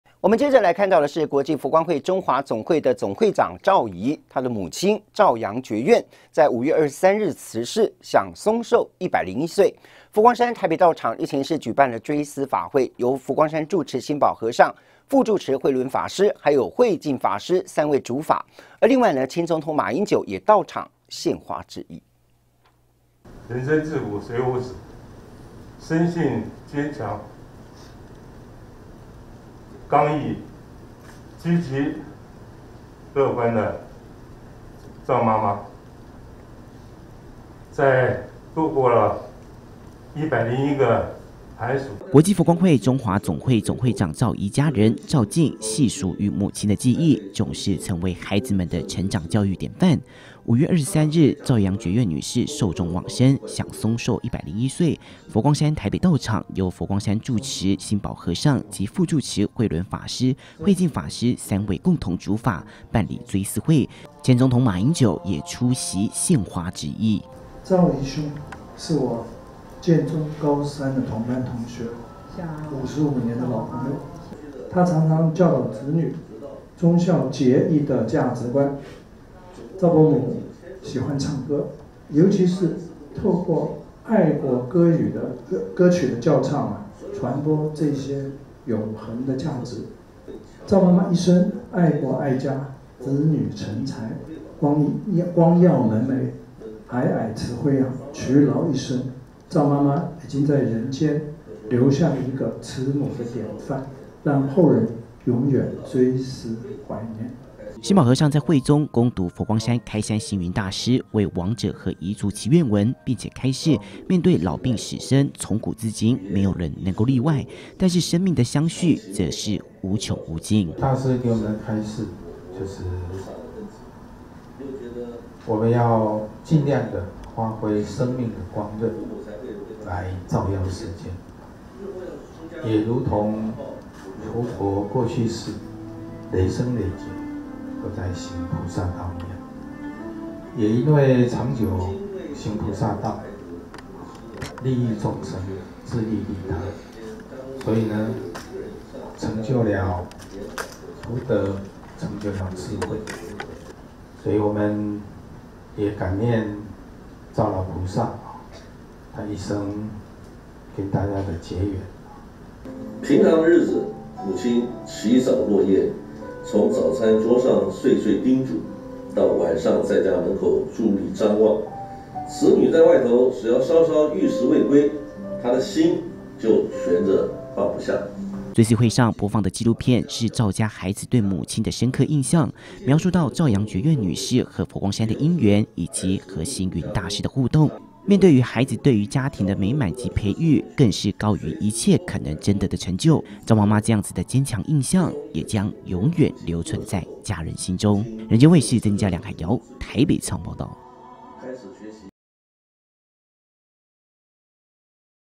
20230606 趙楊覺苑追思法會 馬英九出席緬懷致意｜@FGSBLTV
國際佛光會中華總會總會長趙怡，其母親趙楊覺苑在5月23日辭世，享嵩壽101歲，佛光山台北道場日前舉行追思法會，由佛光山住持心保和尚，副住持慧倫法師及慧靖法師三位主法，前總統馬英九也到場獻花致意，現場肅穆莊嚴。